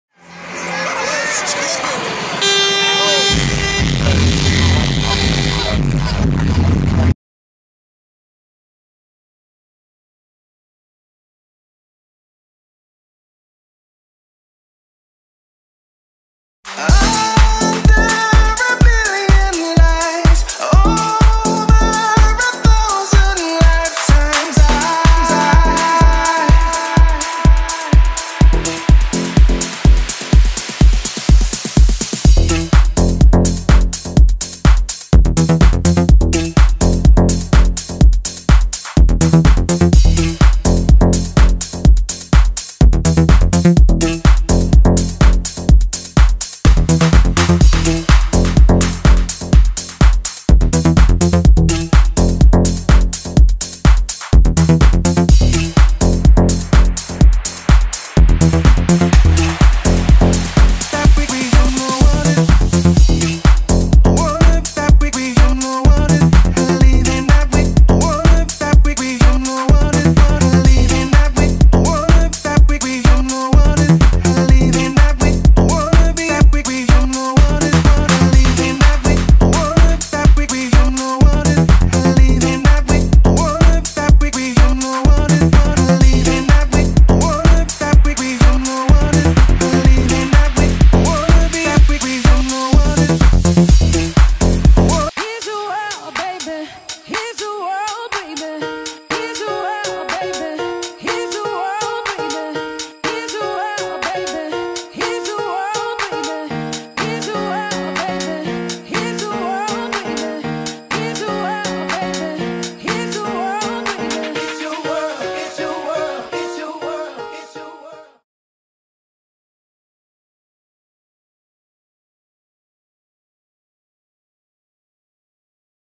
BRIXTON SPLASH - FEAT LEE B3 EDWARDS MARK RADFORD LANCE MORGAN
A few of my favorite Dj's Shutting down Brixton!